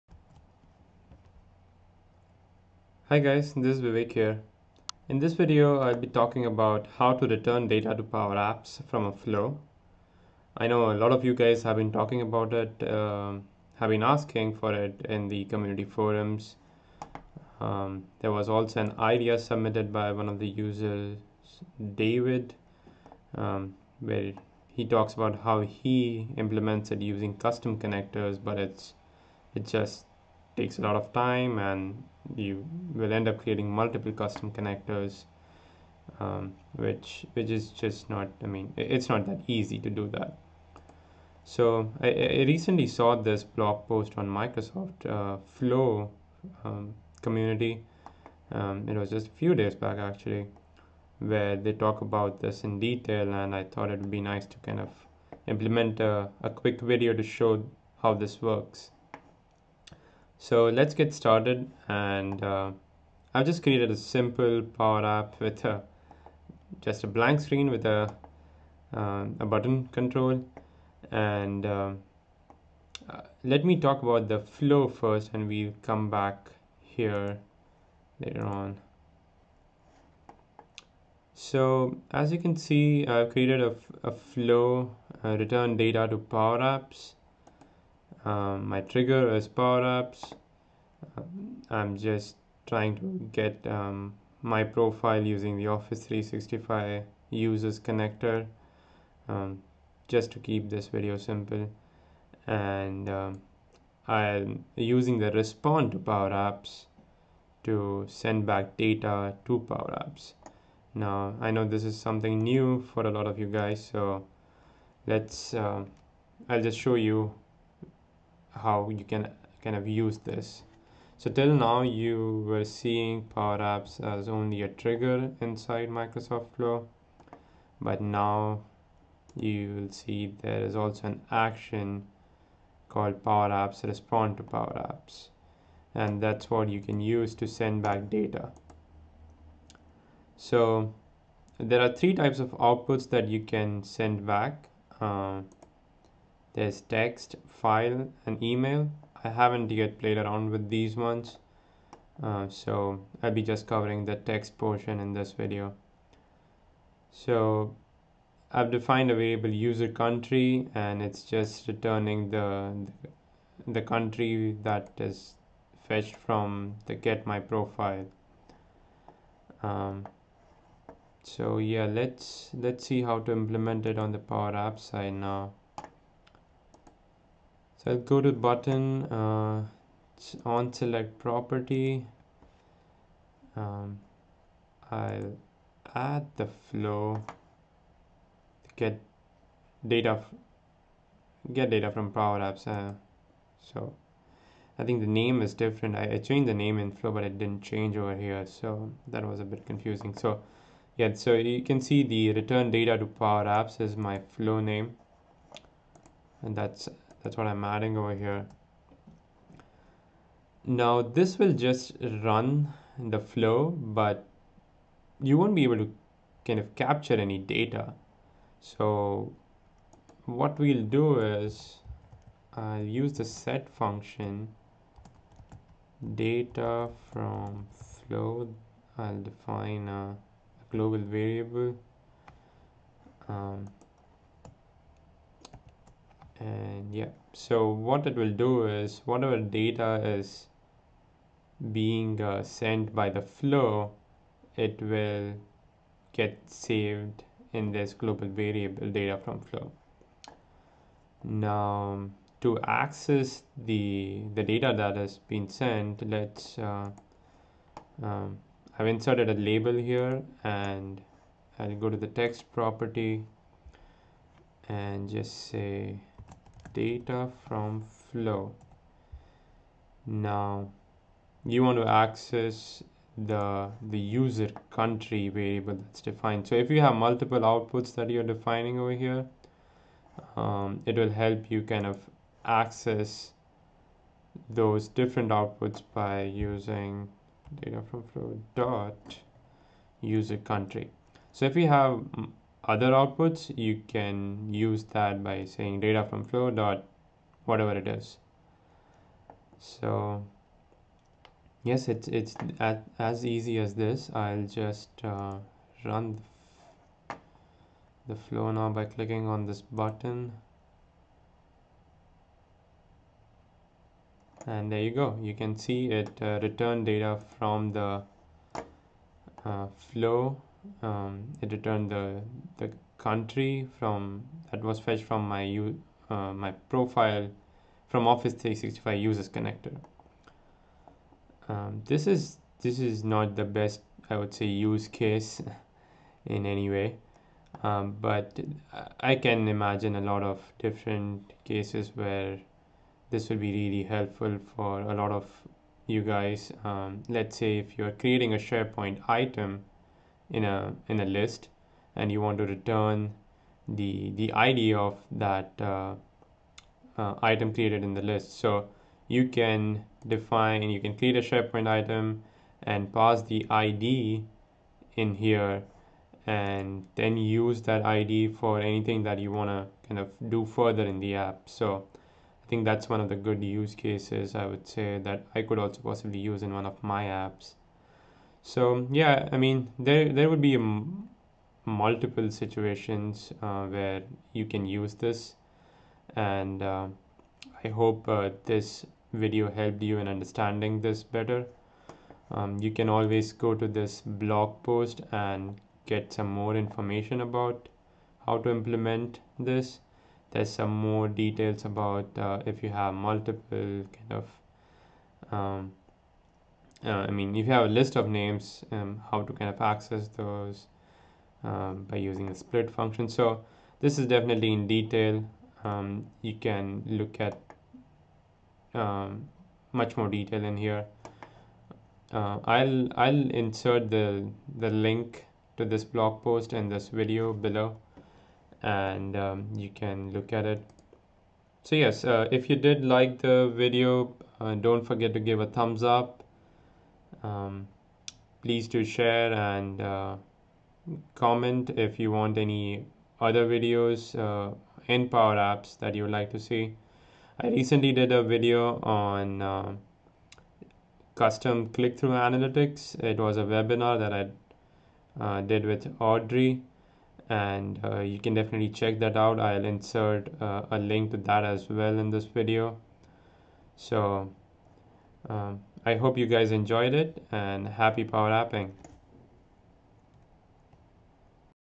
Return data from Flow to PowerApps
In this video, I talk about how to return data from Flow to PowerApps. This is a really useful feature if you need to access data generated in the process of a flow back in PowerApps.

If you are interested in creating Custom Click-through Analytics in your PowerApps, see this webinar / blog post-